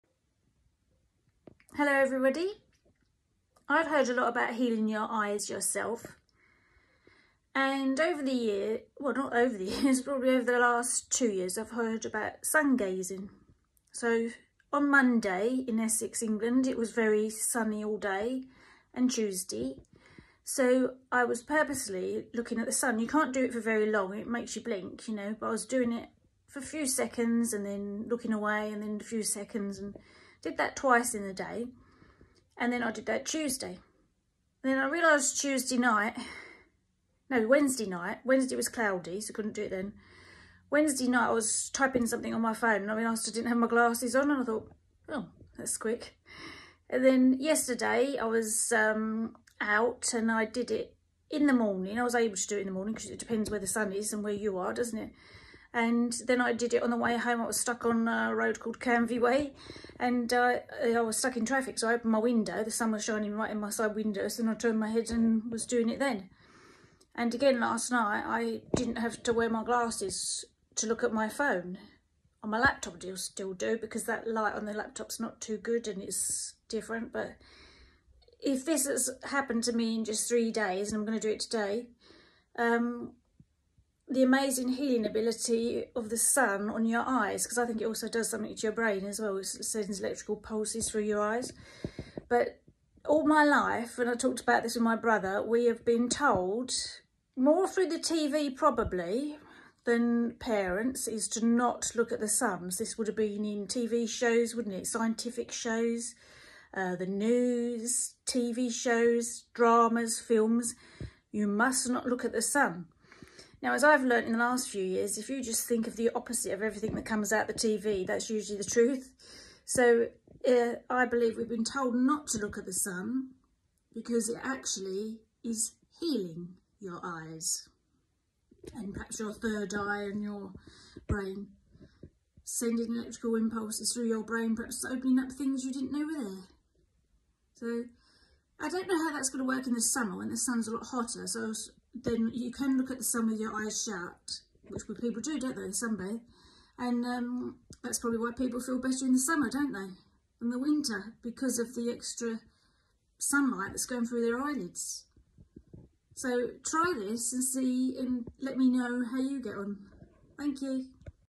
Sun Gazing Is Healing Eyes
Sun Gazing has improved my eyes in two days .